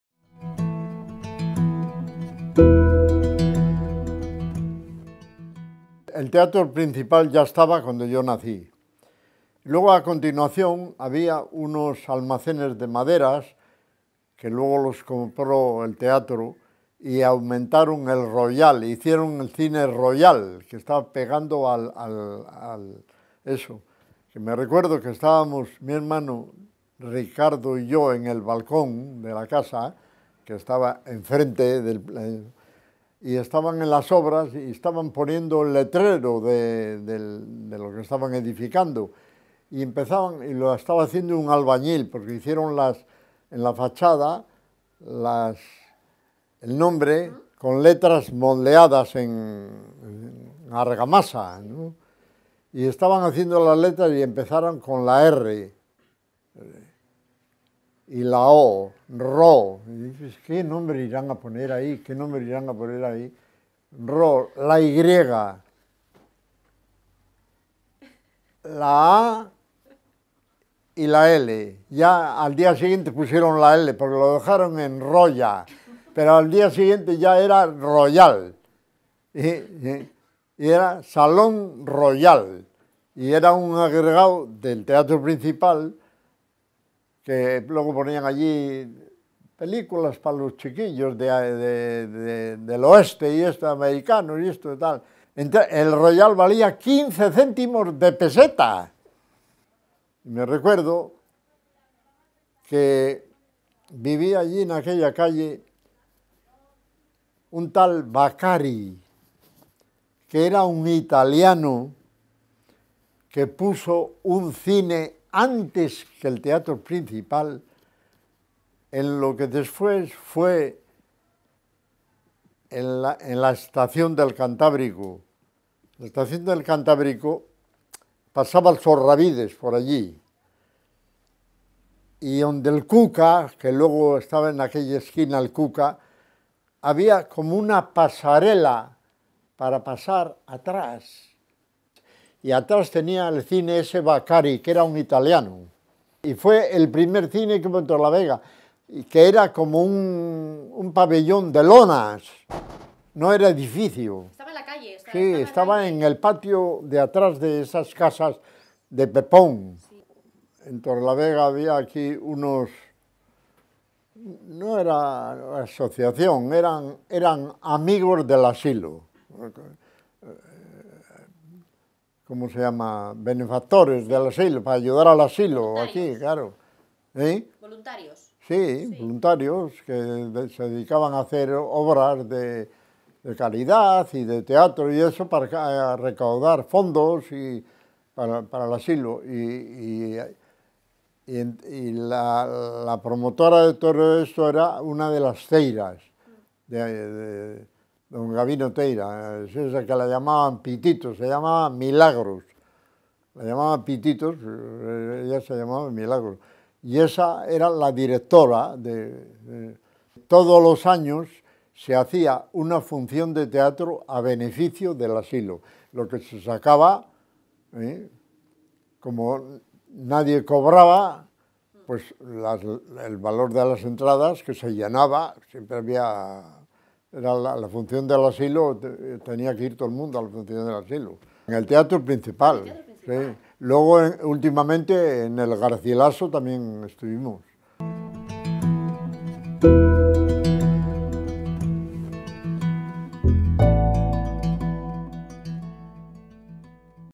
Torrelavega - Teatro Principal - Salón Royal - En Primera Persona Cantabria
En Primera persona. Andrés Argos nos cuenta como eran los teatros y cines de Torrelavega hace más de 70 años.
Tradición oral de Cantabria.